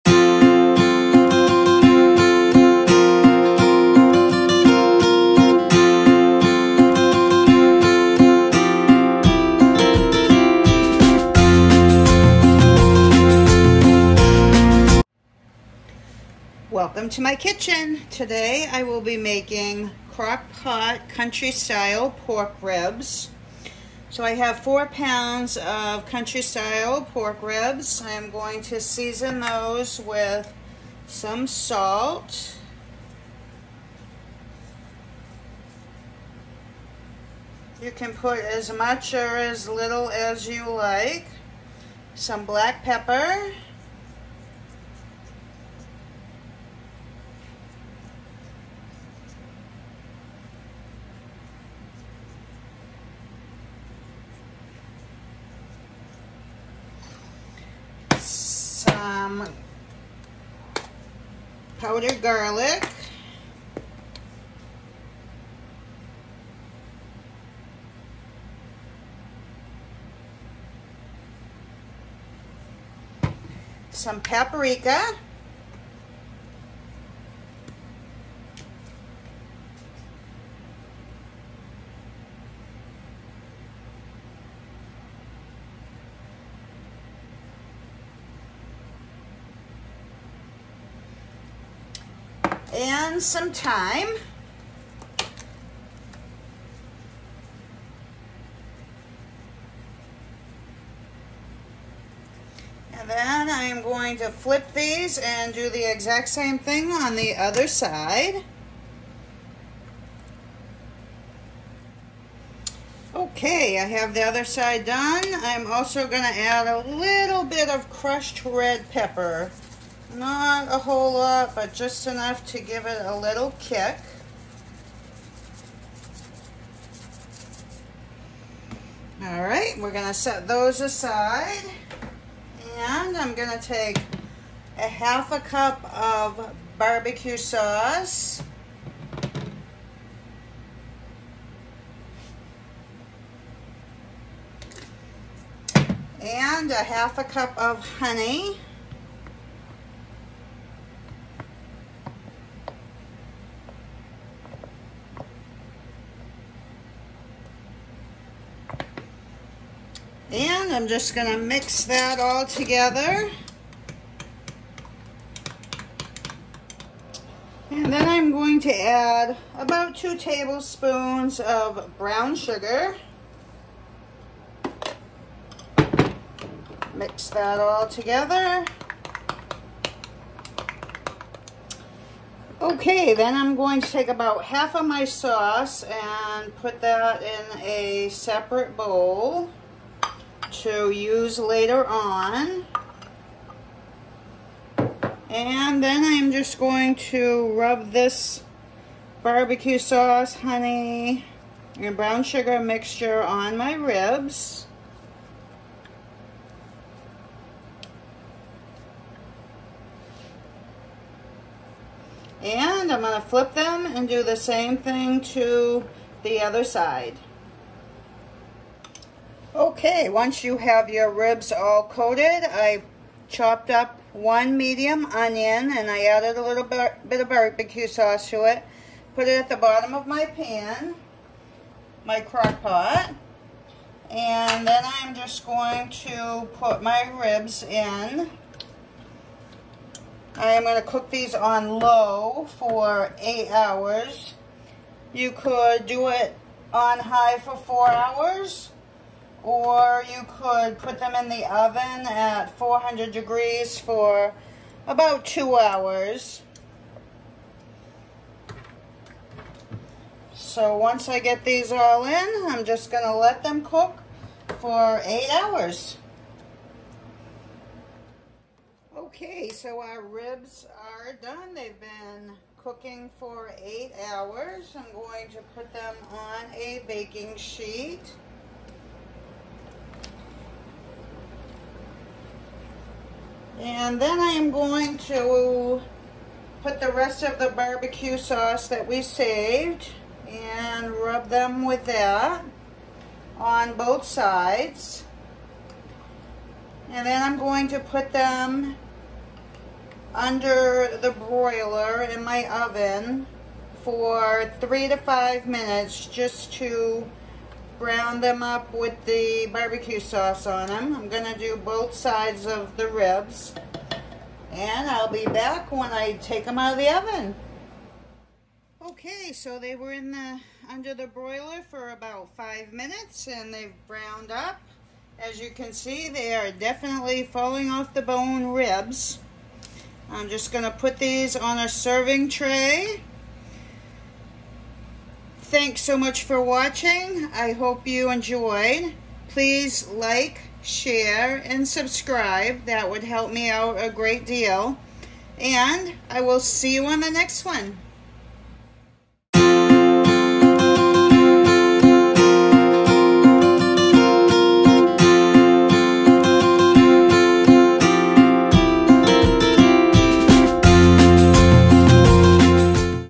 Crockpot Country Style Pork Ribs Recipe ~ Fall Off The Bone!
3-4 pounds country style pork ribs
salt
black pepper
garlic powder
paprika
thyme

Sauce:
1/2 cup barbecue sauce
1/2 cup honey
2 TBS brown sugar

Cook in the crockpot for 8 hours on low, or 4 hours on high. Then place under the broiler in your oven for 3-5 minutes.   You can also put these in the oven at 400 degrees for 2 hours.  Enjoy!